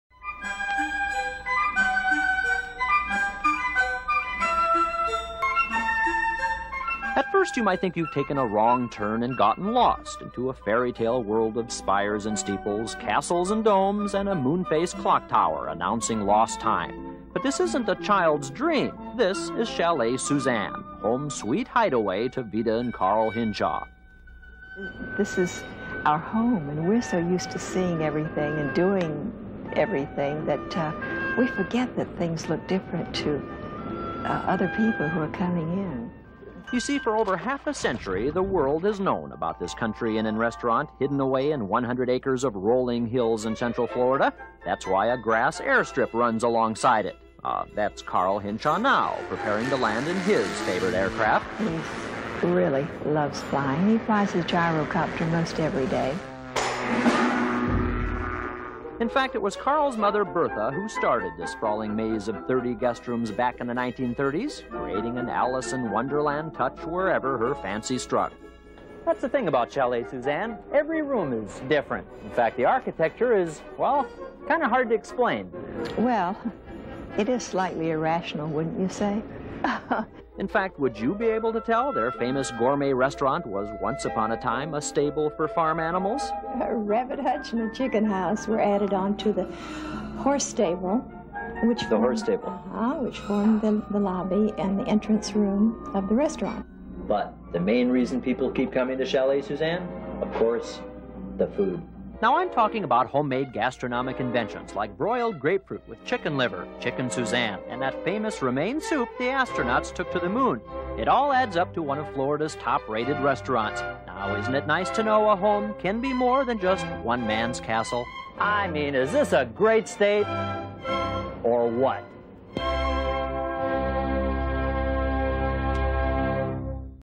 Chalet Suzanne: A Florida Roadside Classic
1987: Home of the Soup That Went to the Moon!

WTVJ-4's John Holden drops into Chalet Suzanne in this 1987 story. This was after the Polk County landmark's trademark Romaine Soup had flown to the moon with U.S. astronauts and after most of it burned down in 1943 and long after it was started as a high-end golf and tennis resort co-founded by cheese baron James L. Kraft and Carl and Bertha Hinshaw.

But it was before the fabled inn and eatery closed in 2014 when its co-owners, the heirs to the Hinshaws retired, and before the Chalet's line of canned soups, introduced in 1956, ceased production, and well before the resort's conversion to a drug and alcohol rehabilitation center.

In its day Chalet Suzanne was known for its gourmet cuisine and its eccentric, pieced-together architecture, a legacy of the 1943 fire. Cans of its Romaine soup flew into space on Apollo 15 & 16, earning it the nickname "Moon Soup."

This clip is derived from news video in the WTVJ Collection. Accession number B00633-15; airdate August 11, 1987.